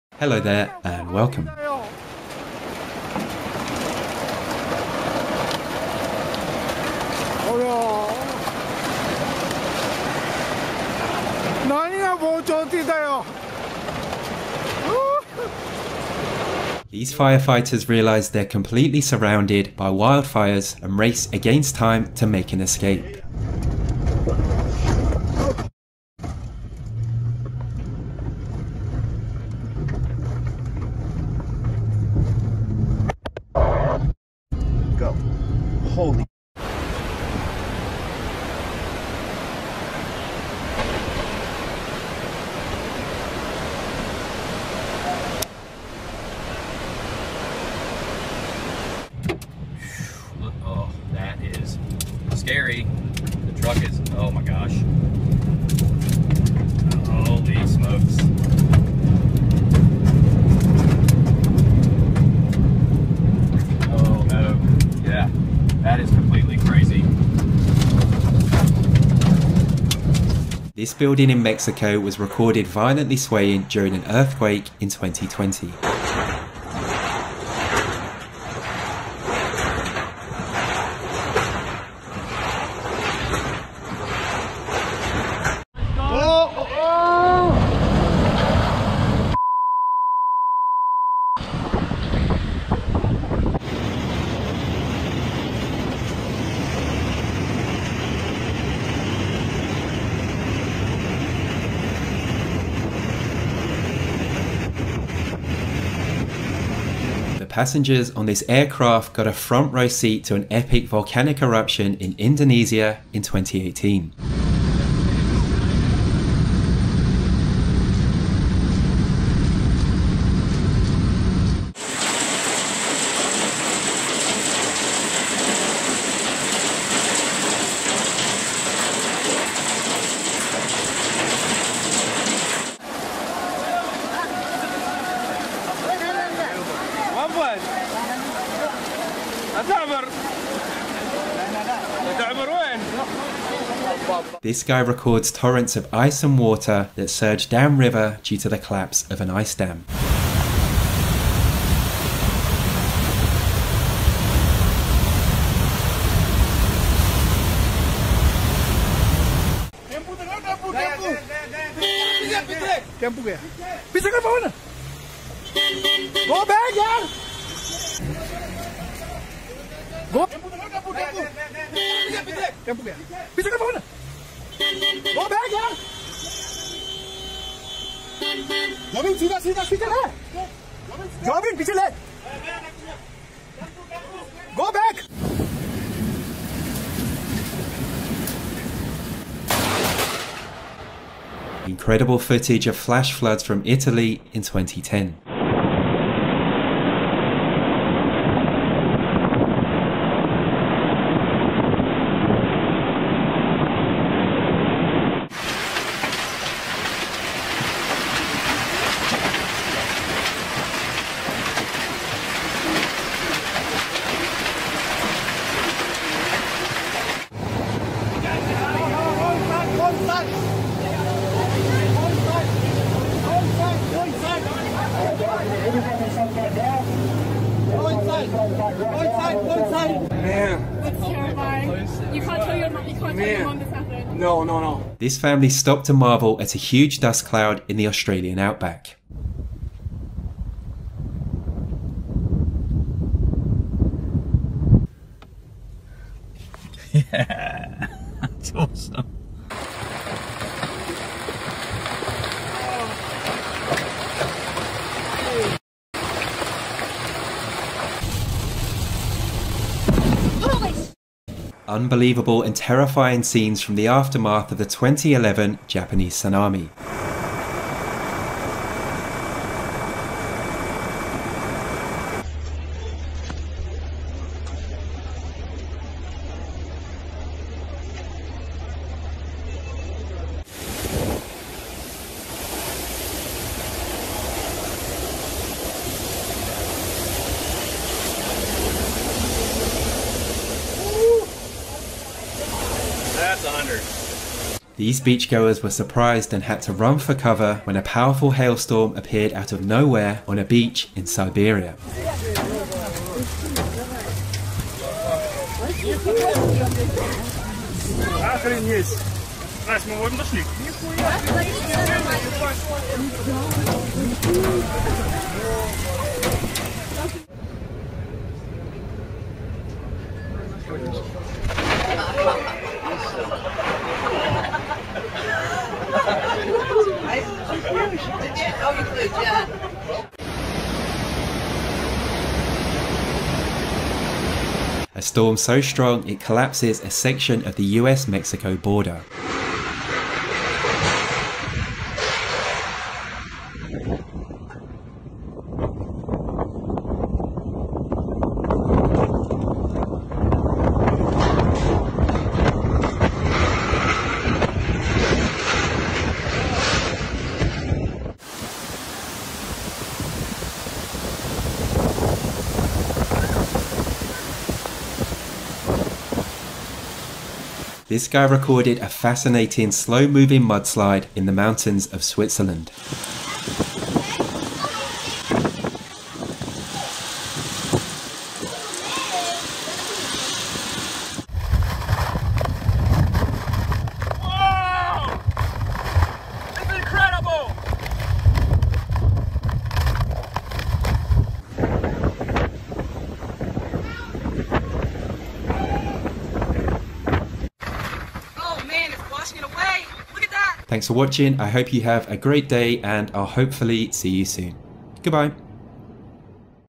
One In a Billion Moments In Nature #6
In this compilation video, we take a look at some of the most extraordinary moments in nature from all around the world.
 Featuring incredible phenomena, to rare natural wonders that are rarely seen.
Sit back as we take a look at one in a billion moments in nature part 2.